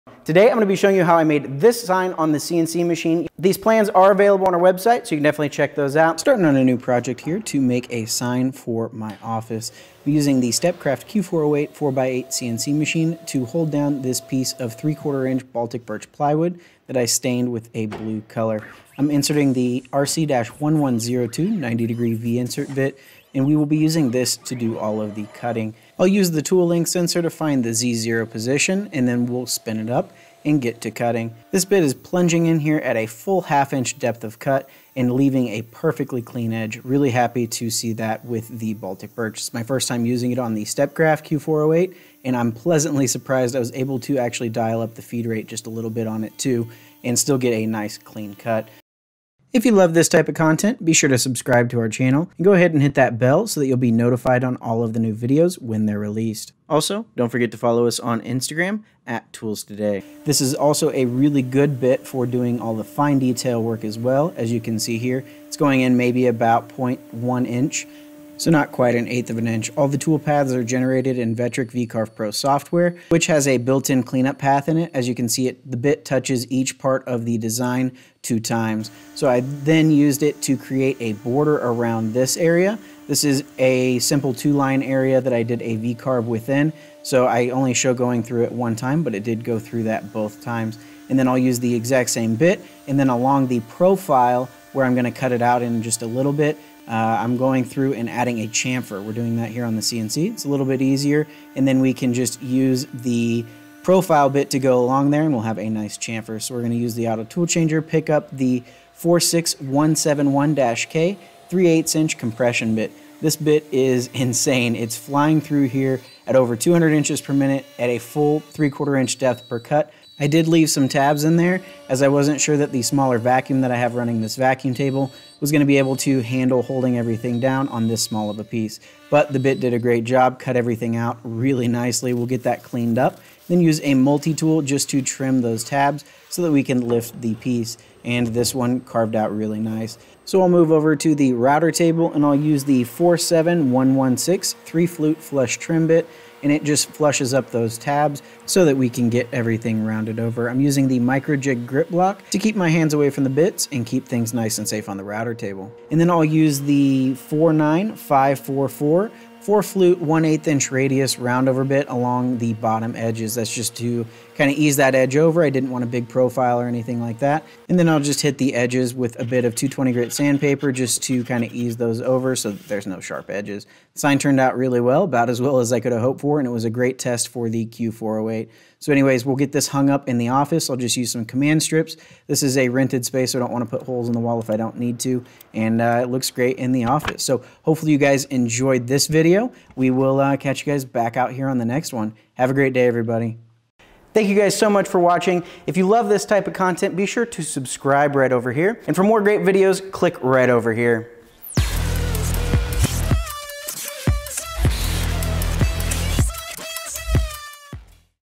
How to Make a CNC Sign | ToolsToday
Making a CNC lake sign using Amana Tool® router bits in my CNC machine and router table! Links to the plans and all tools used are

The CNC work was done on the Stepcraft Q.408 4'x8' CNC machine, available on our website! All of the carving was done using Amana Tool no. RC-1102 90° Insert carbide ‘V’-Groove router bit, this provided a very clean cut especially with the cleanup pass built into the Vectric V-carve pro software available on our website (link below).

Amana Tool RC-1102 CNC 90° Insert V-Groove 1-1/2 D x 3/4 CH x 1/2 Shank Amana Tool Router Bit
RPM: 18,000
Feed Rate: 40ipm
Plunge Rate: 20ipm
Depth per pass: 0.50"

46171-K CNC SC Spektra™ Extreme Tool Life Coated Compression Spiral 3/8 D x 7/8 CH x 3/8 Shank Amana Tool Router Bit
RPM: 18,000
Feed Rate: 200ipm
Plunge Rate: 100ipm
Depth per pass: 0.75"

47116 3-Flute Carbide Tipped Flush Trim 1/2 D x 1/2 CH x 1/4 Inch SHK w/ Lower Ball Bearing Amana Tool Router Bit

49544 1/8 Radius Carbide Tipped 4 Flute Corner Rounding/Beading 7/8 D x 7/16 CH Amana Tool Router Bit